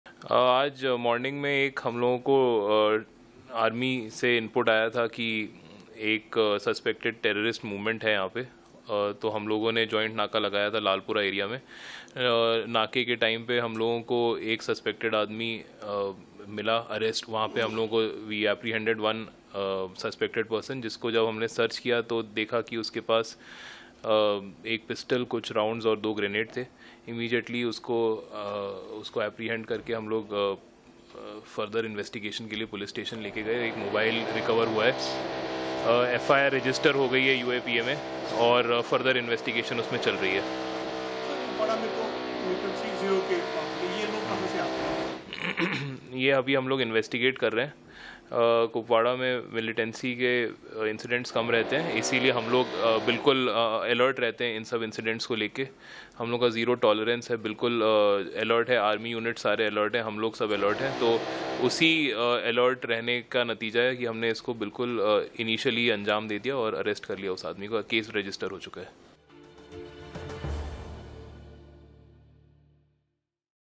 J-K: Terrorist associate along with arms, ammunition arrested in Kupwara; booked under UAPA
Kupwara (J&K): SSP Kupwara Shobhit Saxena on February 15 shared that there is a suspected terrorist movement in the area. He also added that security arrangements have been done in Lalpora area. Shobhit Saxena said, “Today morning, we received input from Army about a suspected terrorist movement here. We put up a joint naka in Lalpora area. We apprehended one suspected person and found one pistol, a few rounds of bullets and a few grenades from him. We took him to the Police station for further investigation. A mobile phone was also recovered. FIR has been registered under UAPA and further investigation is underway.”